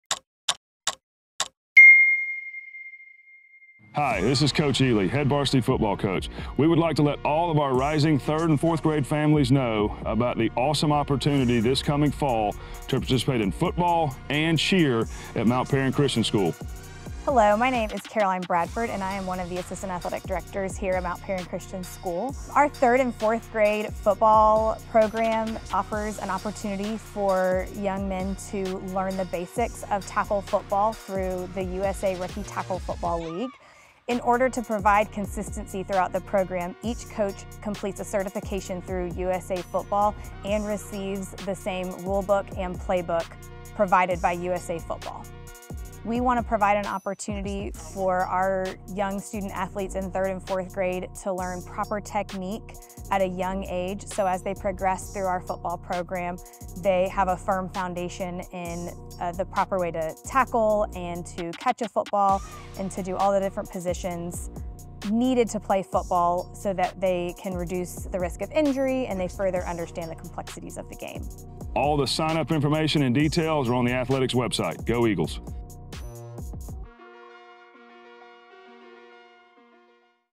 Mount Paran Minute: 3rd and 4th Grade Football
Rising 3rd and 4th grade families have the opportunity to sign their student(s) up for football and cheer for the 2024 fall season. Student-athletes will participate in the USA Tackle Football League led by community coaches that are USA Tackle certified. This is a great way for your student(s) to develop and learn the fundamentals of football.
SYNC ID: MB01IH7GD3EJZEU